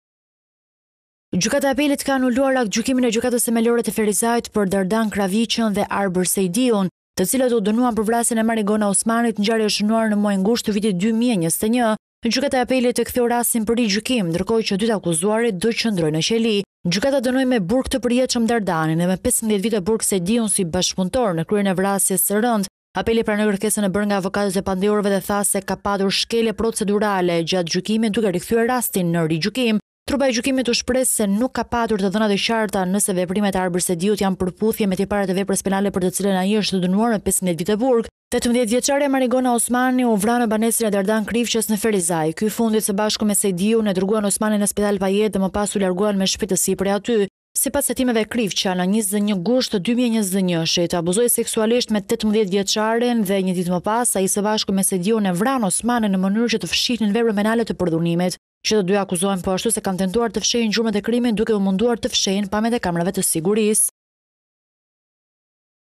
Top News/Përdhunuan dhe vranë 18-vjeçaren/ Kthehet për rigjykim rasti i vrasjes së Marigona Osmanit
Gjykata e Apelit ka anuluar aktgjykimin e Gjykatës Themelore të Ferizajt për Dardan Krivaqën dhe Arbër Sejdiun, të cilët u dënuan për vrasjen e Marigona Osmanit, ngjarje e shënuar në muajin gusht të vitit 2021.
Gjykata e Apelit e ktheu rastin për rigjykim, ndërkohë që dy të akuzuarit, do vijojnë të qëndrojnë në qelinë e paravburgimit.
Gjykata dënoi me burg përjetë Dardanin dhe me 15 vite burg Sejdiun, si bashkëpunëtor në kryerjen e vrasjes së rëndë. Apeli pranoi kërkesën e bërë nga avokatët e të pandehurvre dhe tha se ka pasur shkelje procedurale gjatë gjykimit, duke rikthyer rastin në rigjykim.
Trupa e gjykimit u shpreh se nuk ka patur të dhëna të qarta nëse veprimet e Arbër Sejdiut, janë në përputhje me tiparet e veprës penale për të cilën ai është dënuar me 15 vite burg.
18 vjecarja Marigona Osmaniu vra në banesën e Dardan Krivqës në Ferizaj. Ky i fundit së bashku me Sejdiun e dërguan Osmanin në spital pa jetë dhe më pas u larguan me shpejtësi prej tyre.
Sipas hetimeve, Krivqa në 21 gusht 2021, abuzoi sekuealisht me 18 vjecares dhe se një dit më pas, ai së bashku me Sejdiun e vranë Osmanin, në mënyrë që të fshihnin veprën penale të përdhunimit.
Që të dy akuzoheshin, po ashtu, se kanë tentuar të fshehin gjurmët e krimit, duke u munduar të fshehin pamjet e kamerave të sigurisë.